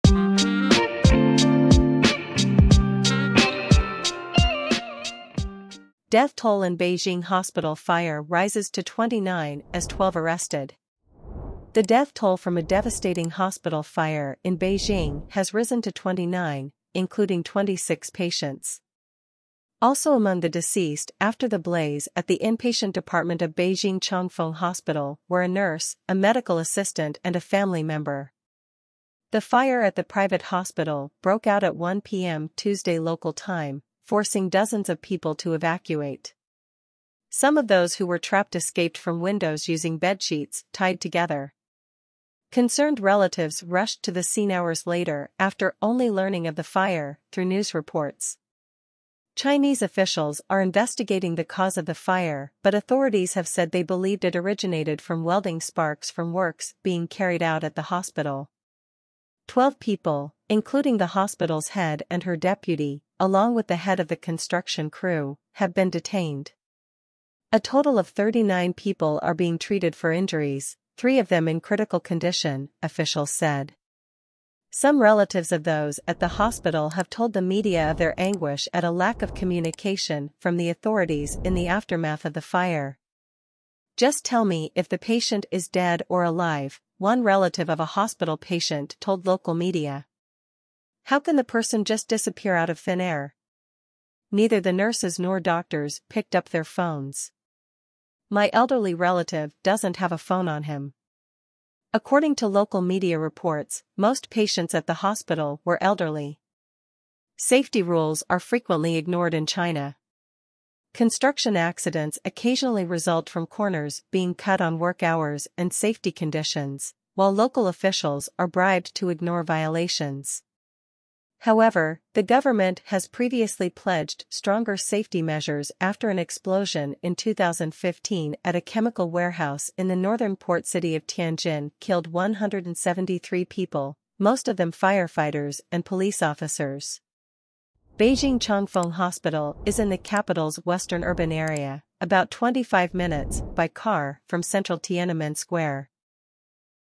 Death toll in Beijing hospital fire rises to 29 as 12 arrested#news
Death toll in Beijing hospital fire rises to 29 as 12 arrested

The death toll from a devastating hospital fire in Beijing has risen to 29, including 26 patients.

Also among the deceased after the blaze at the inpatient department of Beijing Changfeng Hospital were a nurse, a medical assistant and a family member.

The fire at the private hospital broke out at 1pm Tuesday local time, forcing dozens of people to evacuate. Some of those who were trapped escaped from windows using bedsheets tied together.

Concerned relatives rushed to the scene hours later after only learning of the fire through news reports.

Chinese officials are investigating the cause of the fire but authorities have said they believed it originated from welding sparks from works being carried out at the hospital.

12 people, including the hospital’s head and her deputy, along with the head of the construction crew, have been detained.

A total of 39 people are being treated for injuries, three of them in critical condition, officials said.
Some relatives of those at the hospital have told the media of their anguish at a lack of communication from the authorities in the aftermath of the fire.

“Just tell me if the patient is dead or alive,” one relative of a hospital patient told local media.

“How can the person just disappear out of thin air? Neither the nurses nor doctors picked up their phones. My elderly relative doesn’t have a phone on him.”

According to local media reports, most patients at the hospital were elderly.

Safety rules are frequently ignored in China. Construction accidents occasionally result from corners being cut on work hours and safety conditions, while local officials are bribed to ignore violations.

However, the government has previously pledged stronger safety measures after an explosion in 2015 at a chemical warehouse in the northern port city of Tianjin killed 173 people, most of them firefighters and police officers.

Beijing Changfeng Hospital is in the capital’s western urban area, about 25 minutes by car from central Tiananmen Square.